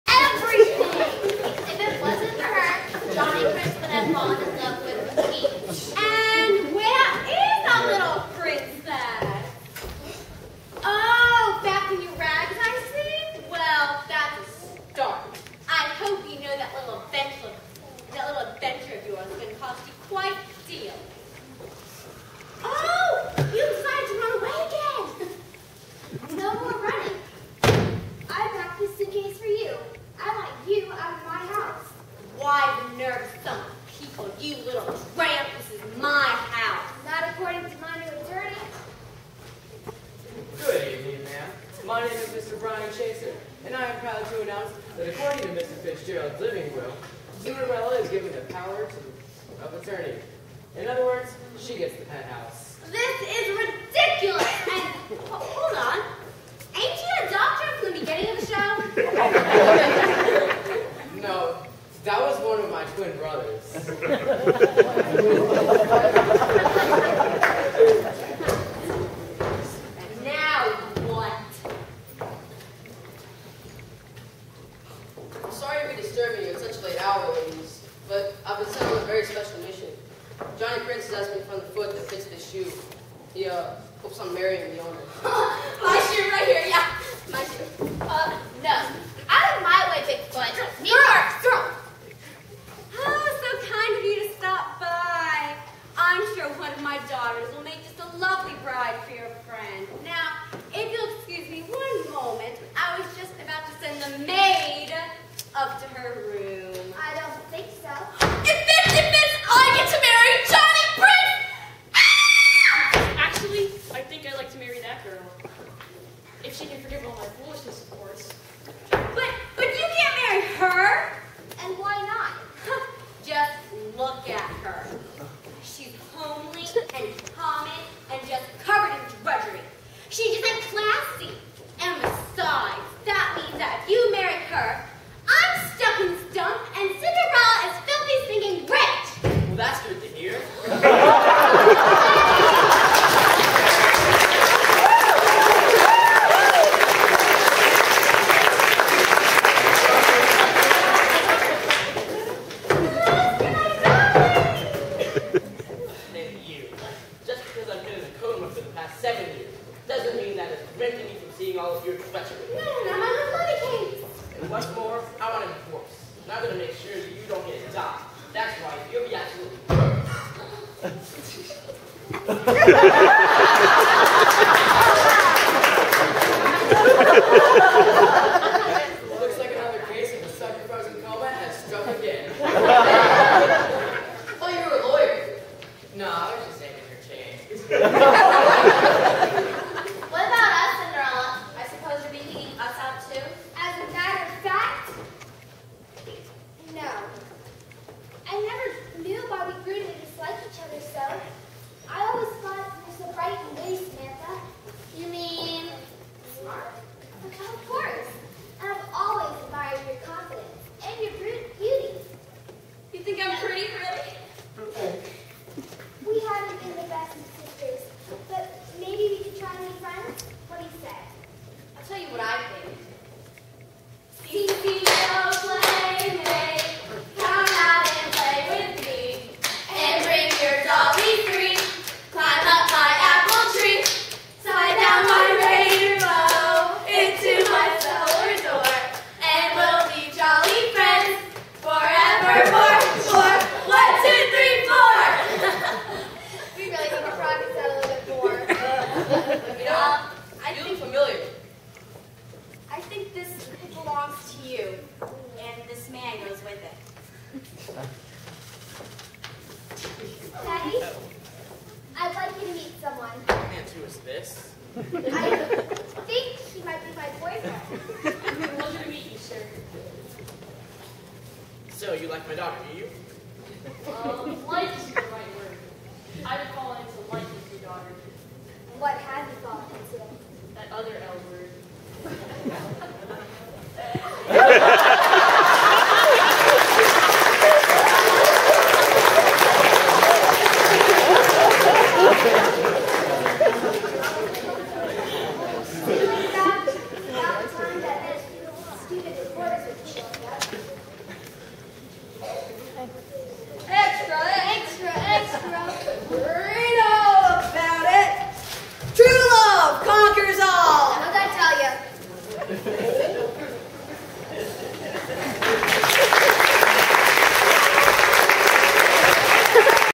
Patrick Hall as Johnny Prince (13 of 14)
(13 of 14) Patrick Hall in Cinderella in New York. Swift Creek Middle School, Apil 2010